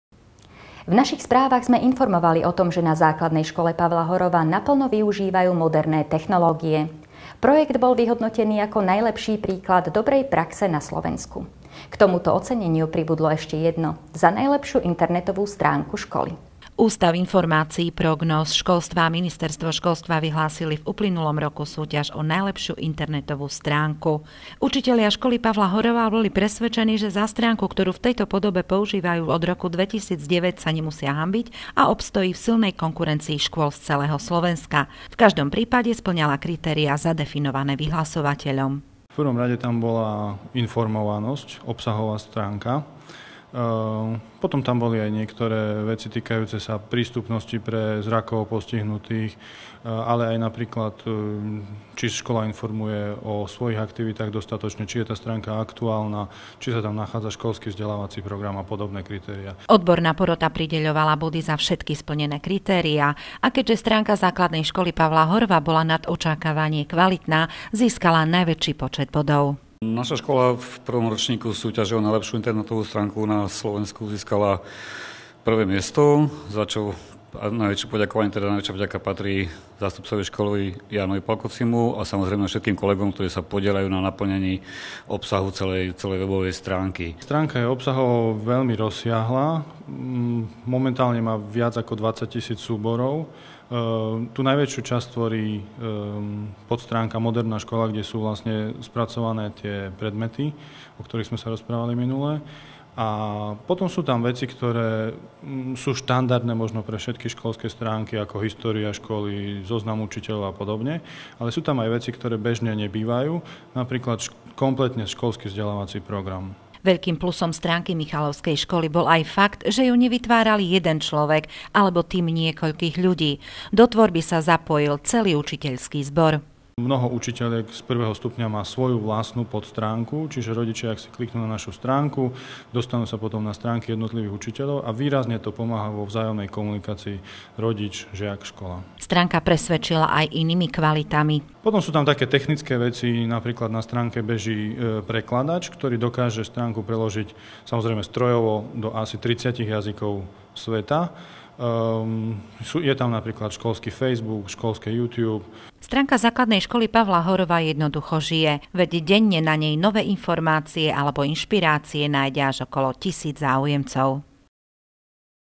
08_Najlepsia_webstranka.mp4
Reportáž TV Mistrál odvysielaná 8.3.2012 o našom víťazstve v súťaži o najlepšiu internetovú stránku školy v kategórie ZŠ.